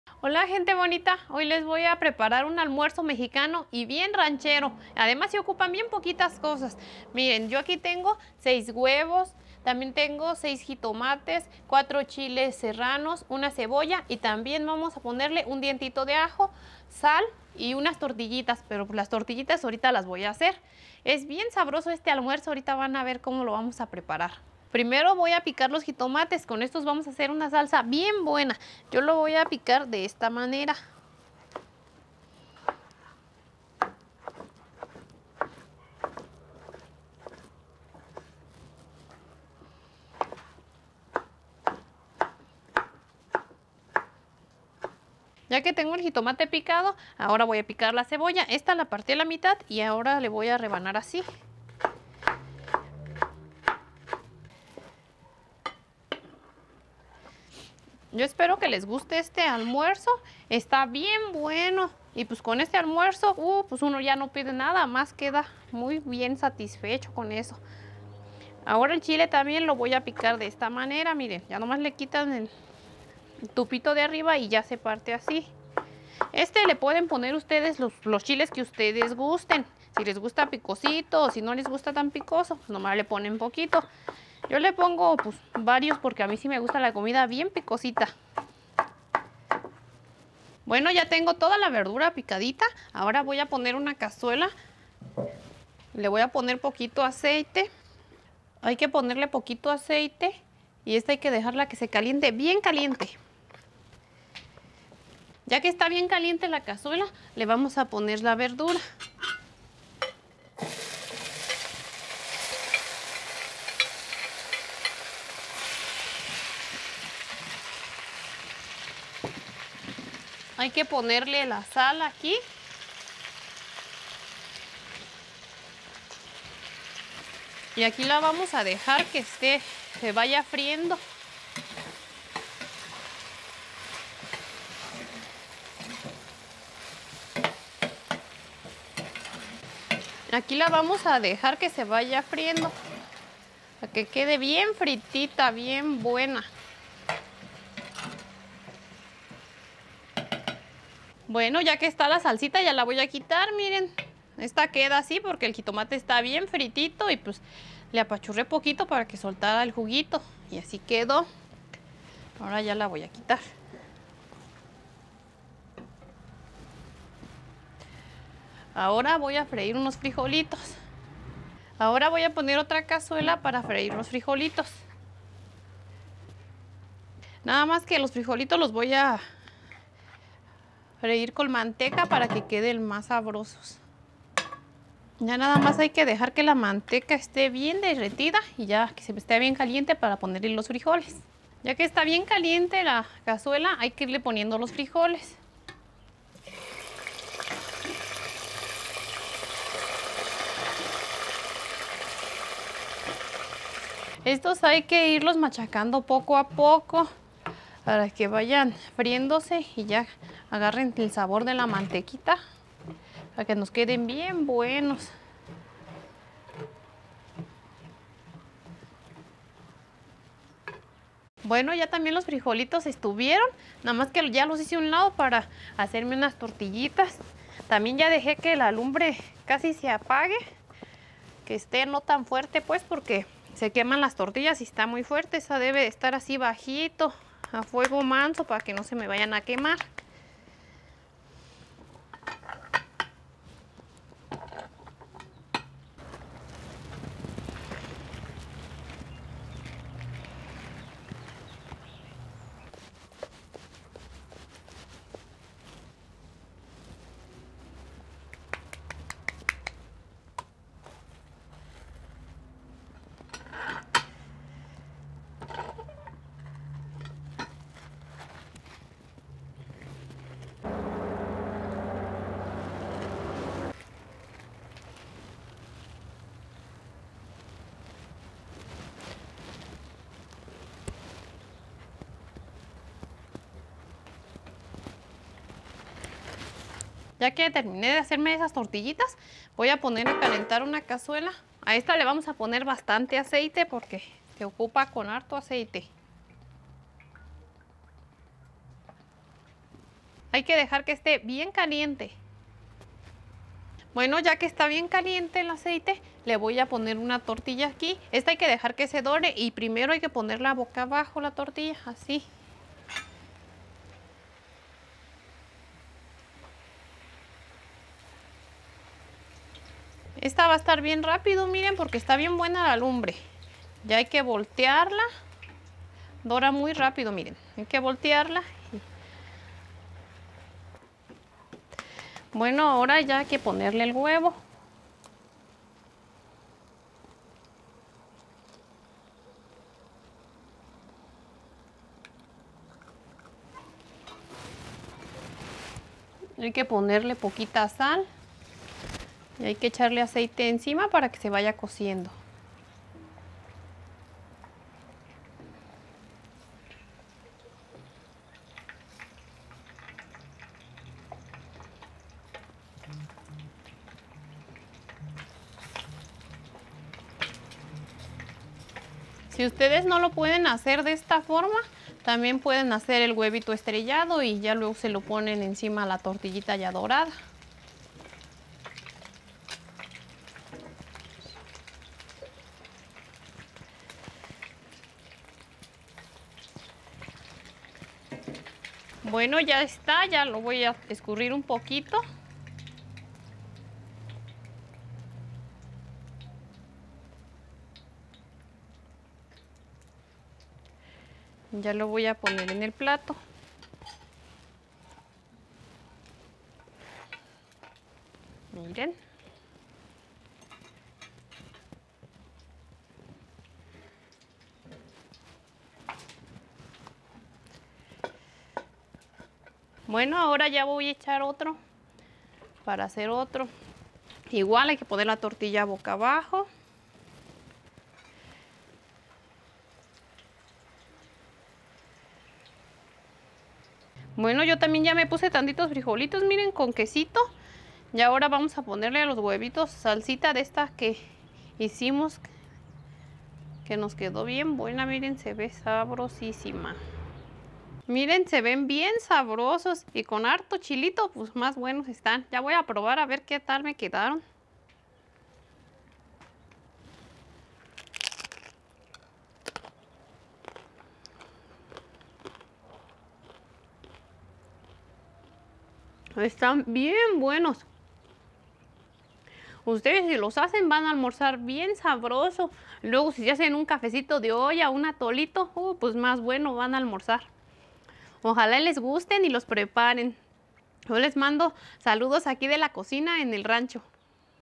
Almuerzo Mexicano De Rancho Así Cocino En El Rancho
Almuerzo Mexicano De Rancho La Cocina En El Rancho mi gente bonita este almuerzo de rancho les va a gustar mucho es como hacer unos huevos ranchero como yo los hago en el rancho y quedan muy buenos

Gracias por todo su apoyo mi gente bonita